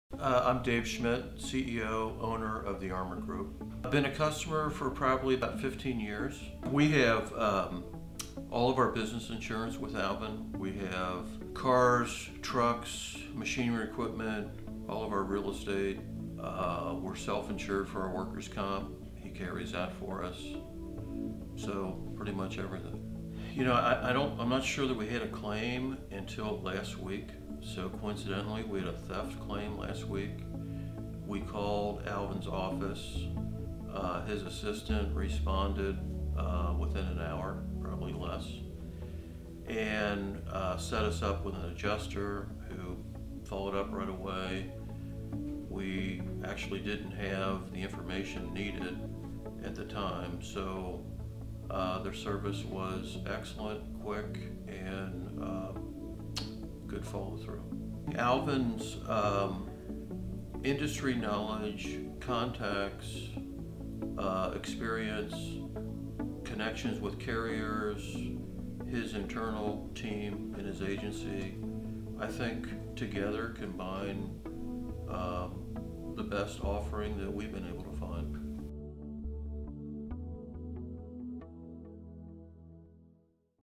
Dave Schmitt - Armor Group | Roehr Client
A testimonial from a long-standing and much-valued Roehr Insurance client.